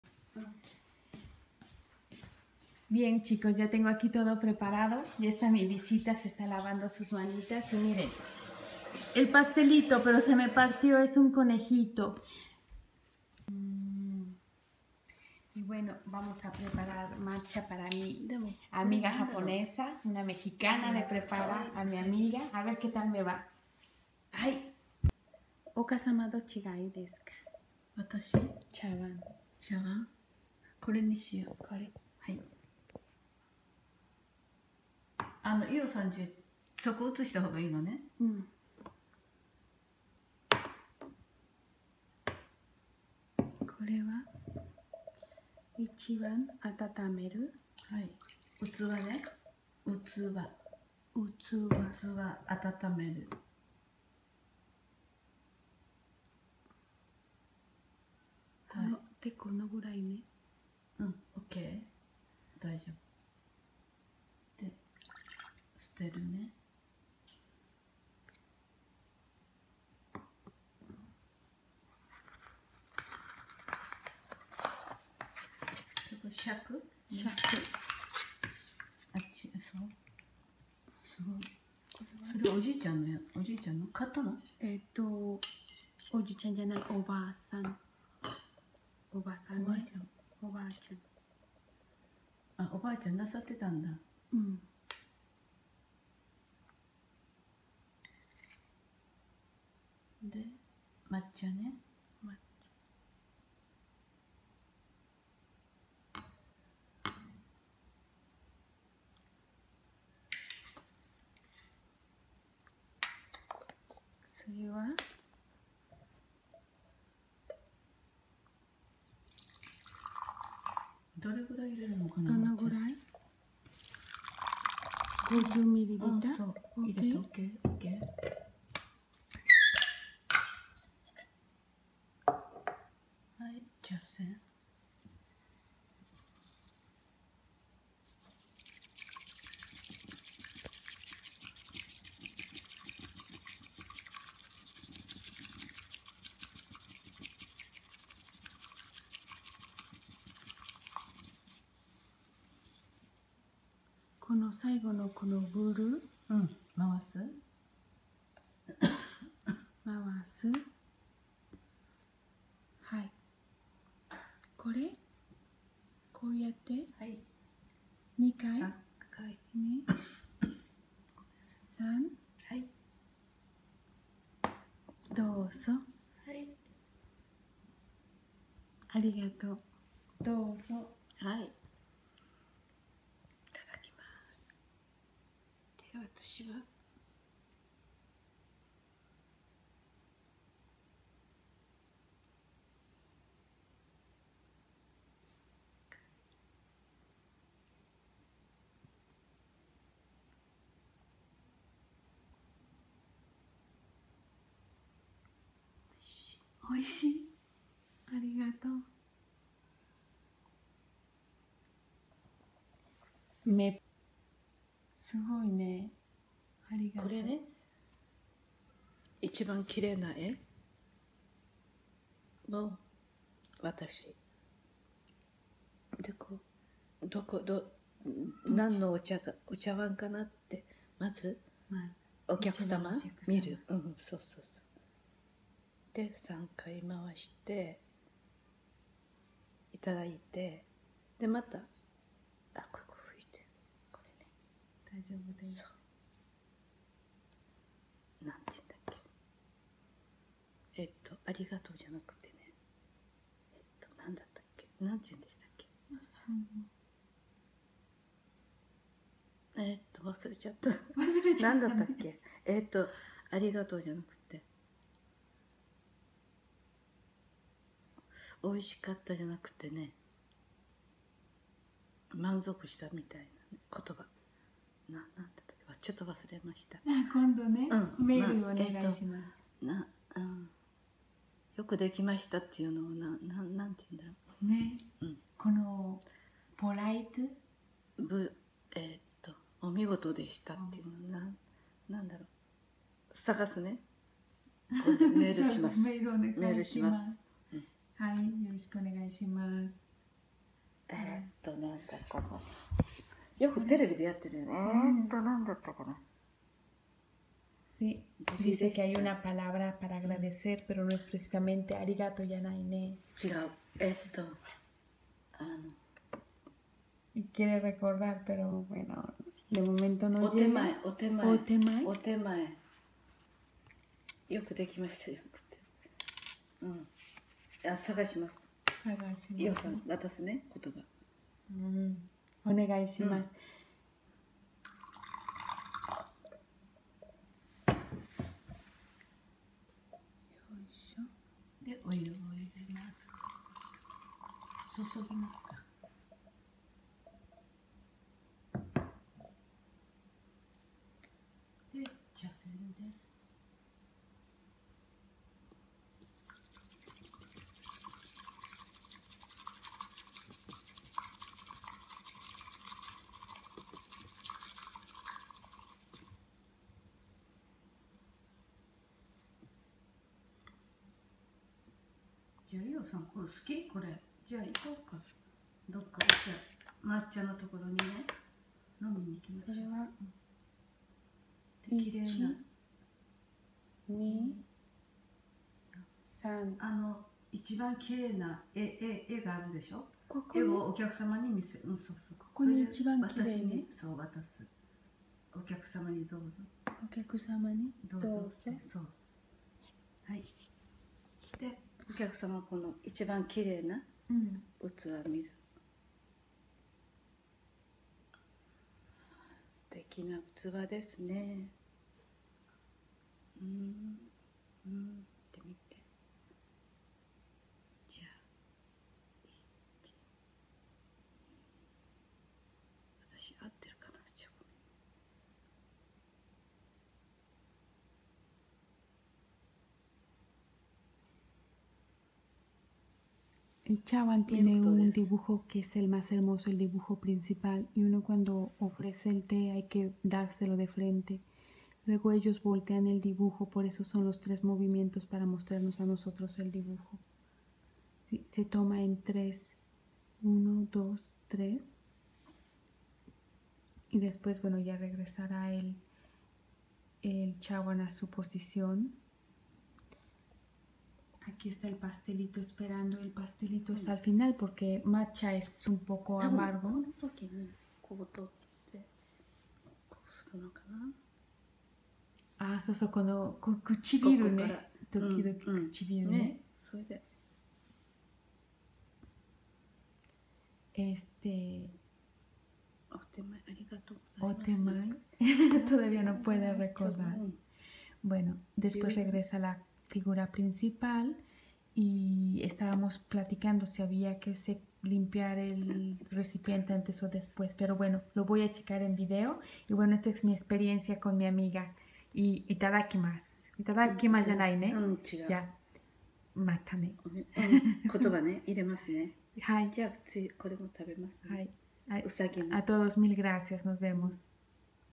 Mexicana en Japón, prepara matcha a su amiga japonesa. Antropóloga En Japón ama la amistad
Aqui chicos!... aqui están los videos cortitos y hermosos hechos por un profesional.